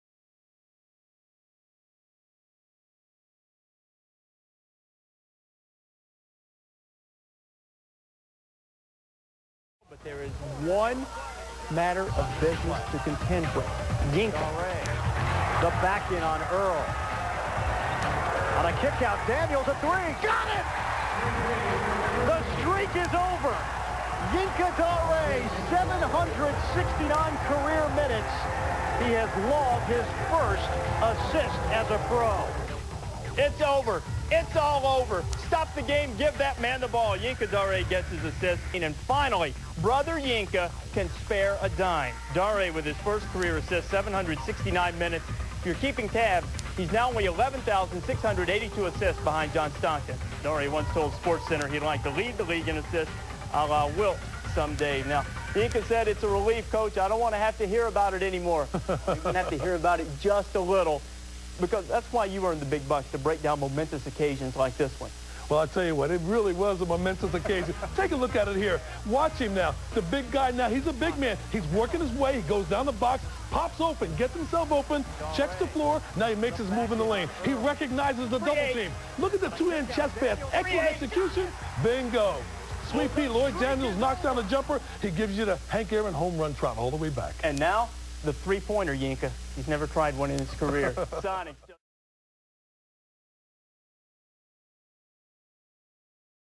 Yinka Dare's First Career Assist
Check out this blast from the past when Yinka Dare finally records his first career assist. check out who made the shot as well!